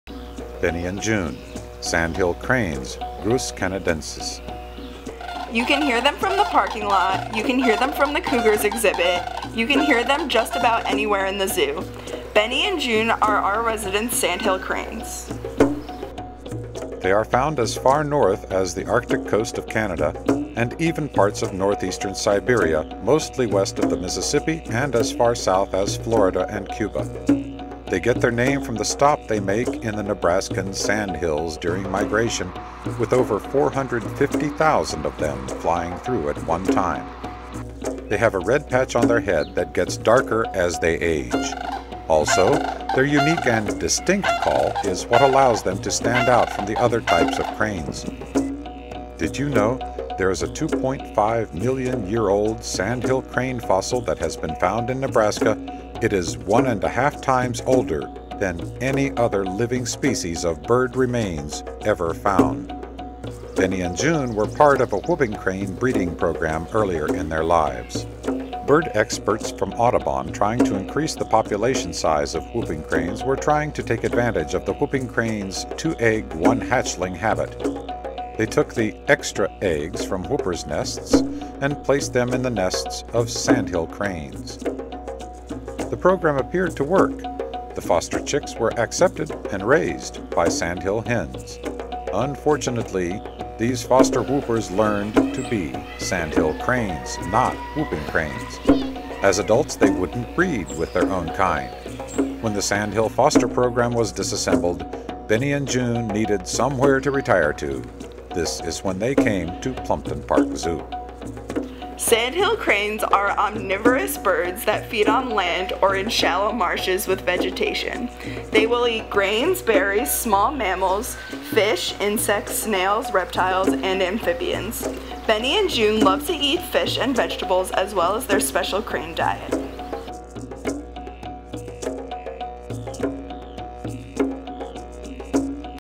Benny and June our Sandhill Cranes - Plumpton Park Zoo
Plumpton Park Zoo presents an educational video about Benny and June, our Sandhill Cranes.